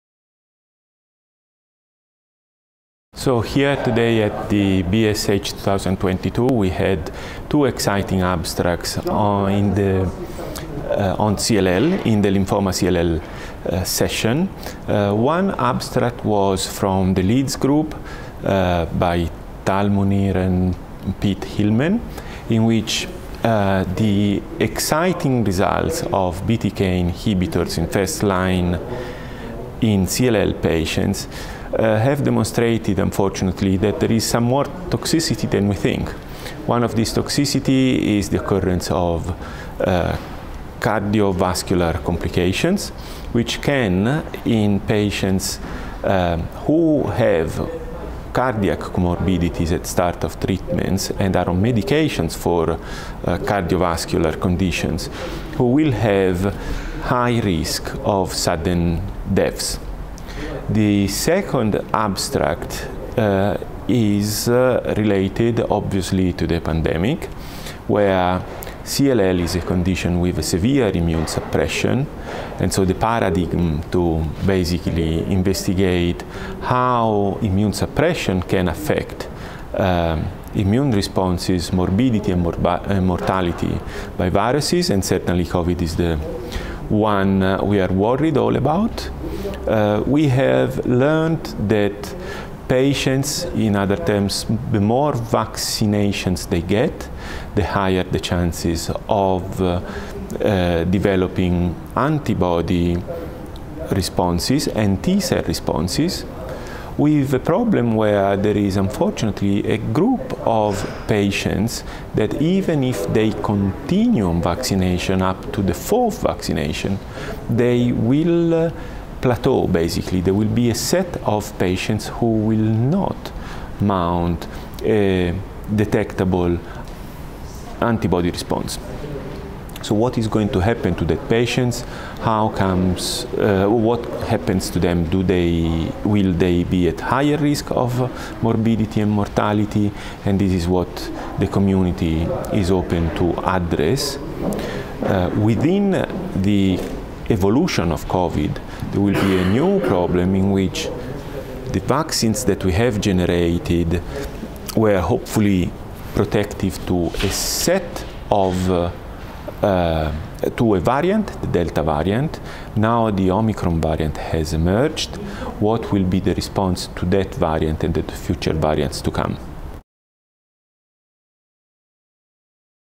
Highlights from the lymphoma and CLL session
Francesco Forconi, MD, DM, PhD, FRCPath, University of Southampton, Southampton, UK, gives an overview of two abstracts presented at BSH 2022 in the lymphoma and chronic lymphocytic leukemia (CLL) session. Dr Forconi first discusses an abstract presented by Dr Talha Munir and Prof. Peter Hillmen on sudden death and higher rates of cardiac toxicity associated with Bruton’s tyrosine kinase (BTK) inhibitors. Dr Forconi then highlights another session which focused on the effects of the COVID-19 pandemic on CLL patients, and further discusses vaccine responses in these immunosuppressed patients. This interview took place at the 62nd Annual Scientific Meeting of the British Society for Haematology (BSH) 2022, in Manchester, UK.